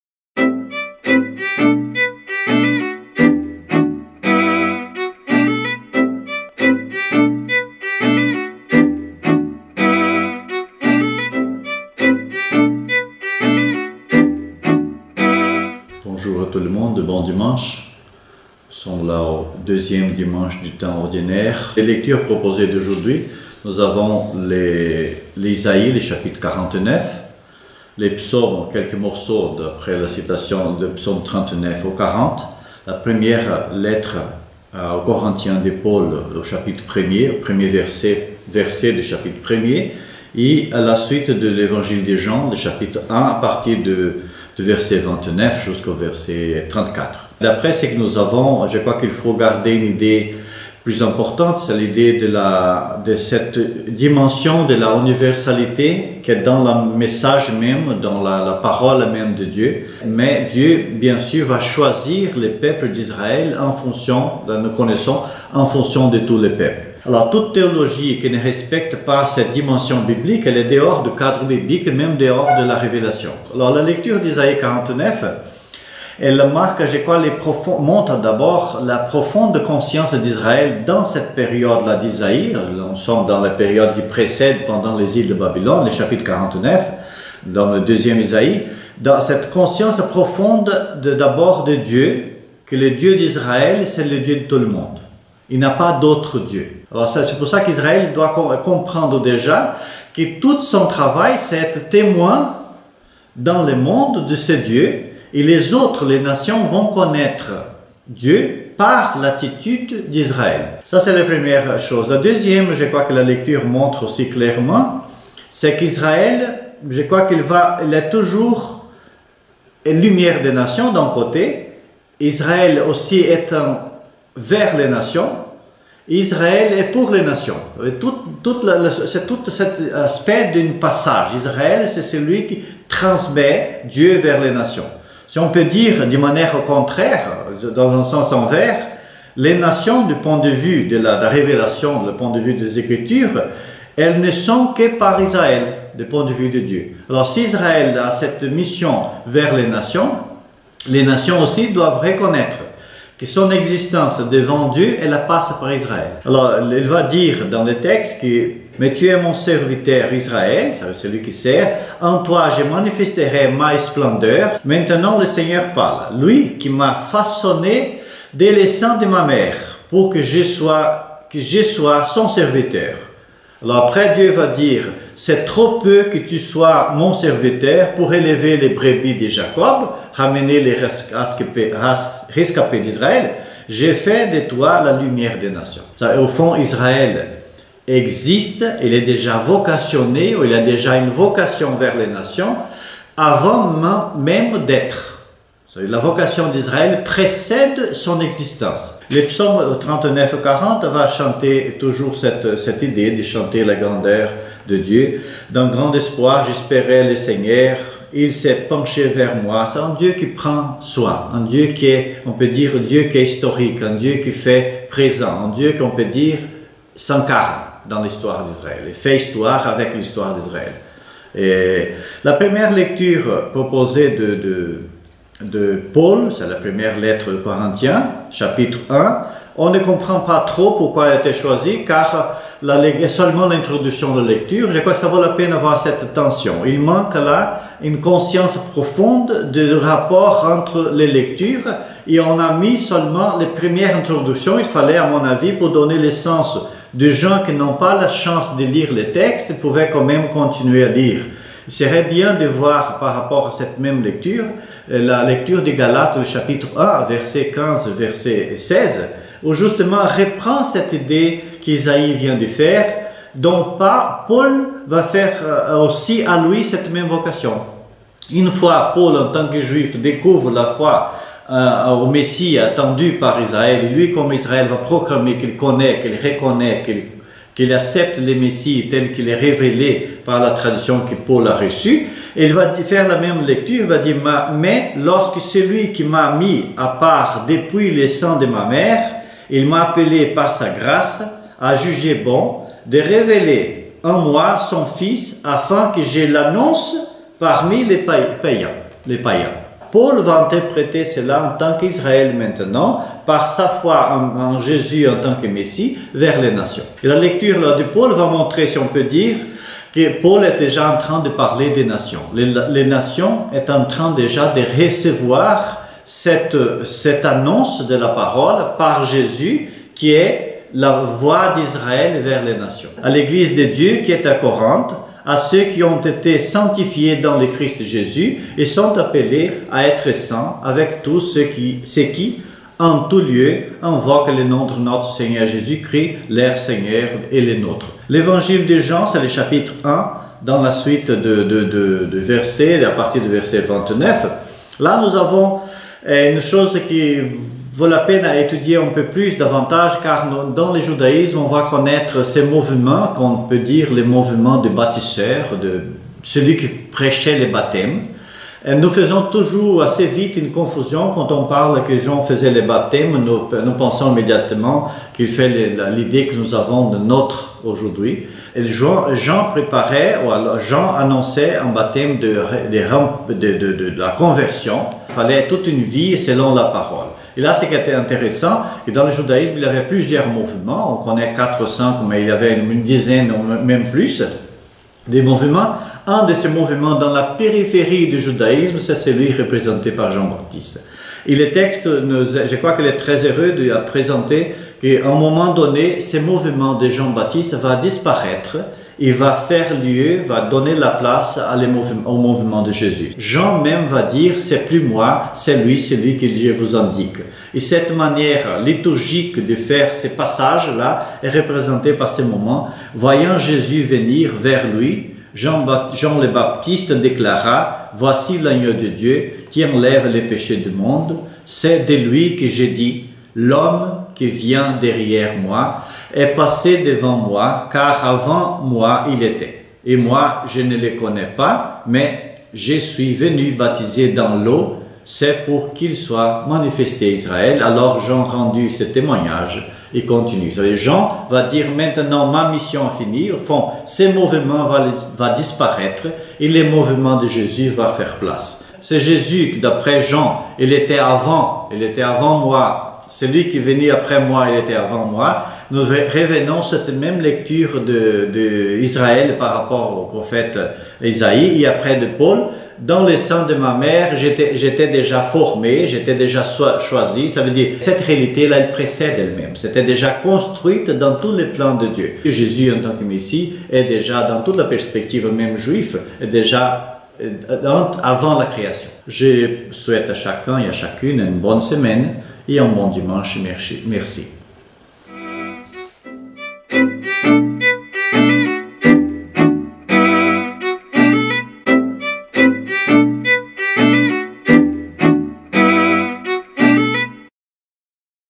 Second Sunday of Ordinary Time (year A).
Commentary on the readings of the Second Sunday of Ordinary Time (15/01/2017), by Br. Elio Passeto, NDS (Brother of Our Lady of Sion).

Audio: French
Subtilte: English/ Portuguese
Images/Edition: Br. Joel Moreira, NDS.